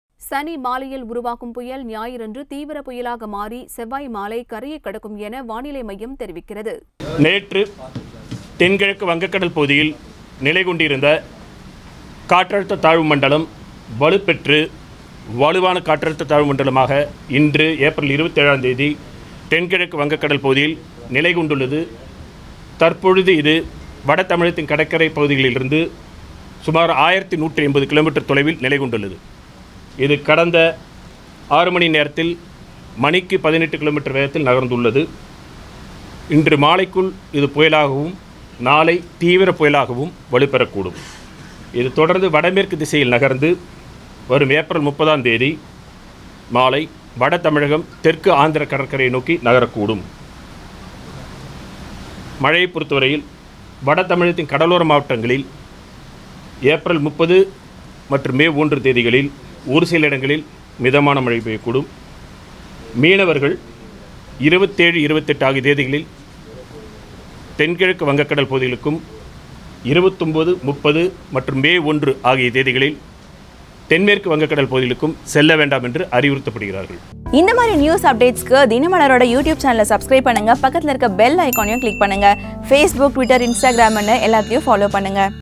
புயலை சந்திக்க ரெடியா?
சனி மாலையில் உருவாகும் புயல்
ஞாயிறன்று தீவிர புயலாக மாறி
செவ்வாய் மாலை கரையை கடக்கும்
என வானிலை மையம் தெரிவிக்கிறது.